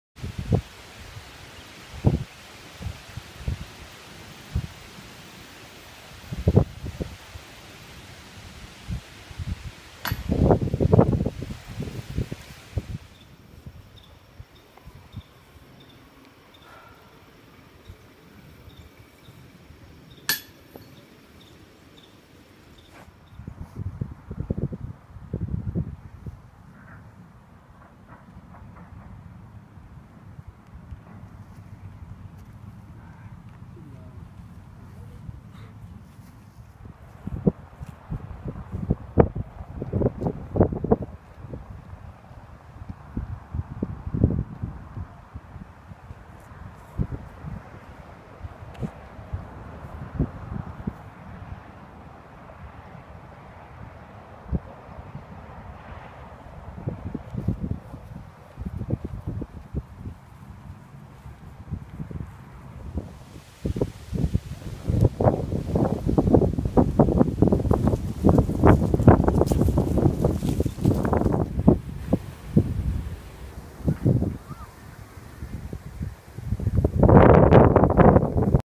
USDGC 2014, 4th Round. Missed birdies.
My missed birdie putts of 4th round. The huge round is usually just inches away.